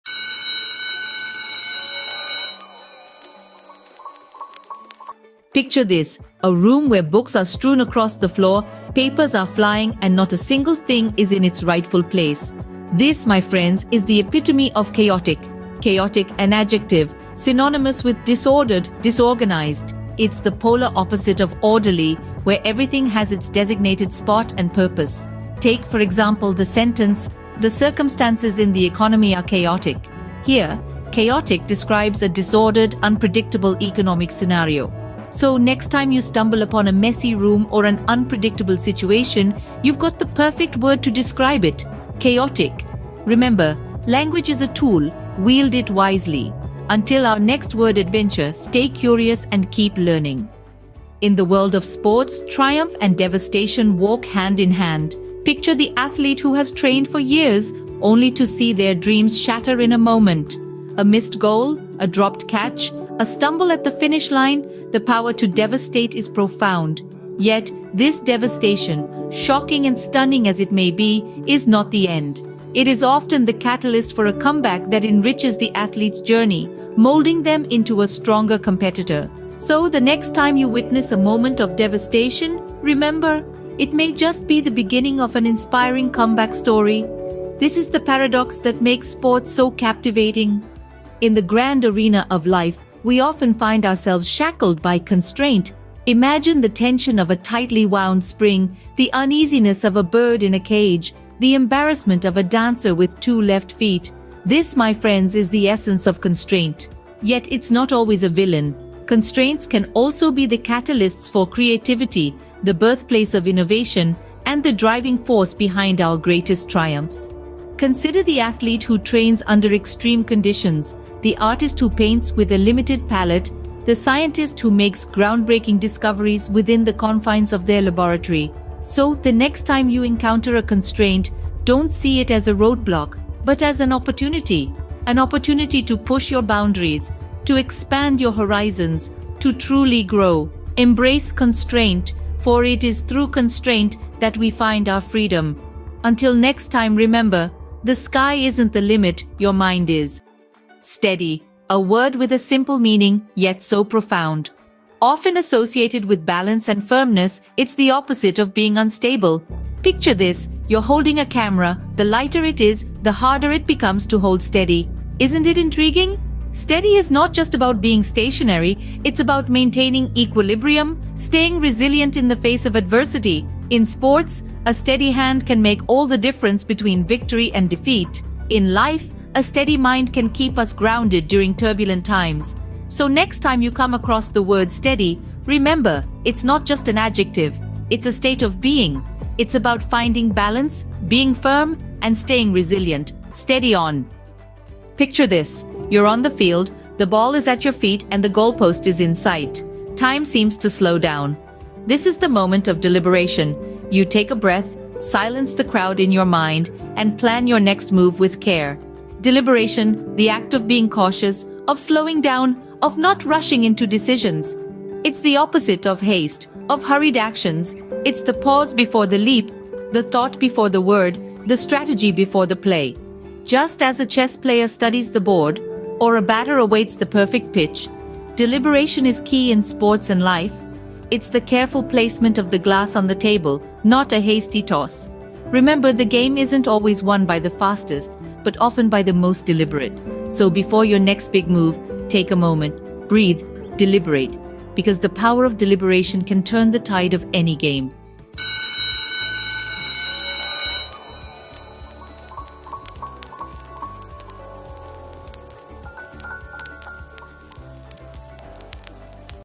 Vocabulary dose | Episode : 18 | Brain Begins
Discover the importance of vocabulary in mastering English! Expand your vocabulary to improve communication, comprehension, reading and writing skills. Gain confidence, fluency, and cultural insights while enhancing learning efficiency. Take a significant step towards becoming a proficient English speaker.

CHAOTIC (ADJECTIVE): (अस्तव्यस्त): disorderly
Synonyms: disordered, disorganized
Antonyms: orderly
Example Sentence:
The circumstances in the economy are chaotic.

DEVASTATE (VERB): (तबाह करना): shatter
Synonyms: shock, stun
Antonyms: enrich
Example Sentence:
She was entirely devastated due to the loss of her soon.

CONSTRAINT (NOUN): (बाध्यता): inhibition
Synonyms: uneasiness, embarrassment
Antonyms: openness
Example Sentence:
They would be able to talk without constraint.

STEADY (ADJECTIVE): (स्थिर): stable
Synonyms: balanced, firm
Antonyms: unstable
Example Sentence:
The lighter the camera, the harder it is to hold steady.

DELIBERATION (NOUN): (सावधानी): care
Synonyms: caution, slowness
Antonyms: haste
Example Sentence:
He replaced the glass on the table with deliberation.

How to improve English communication?
How to improve Vocabulary?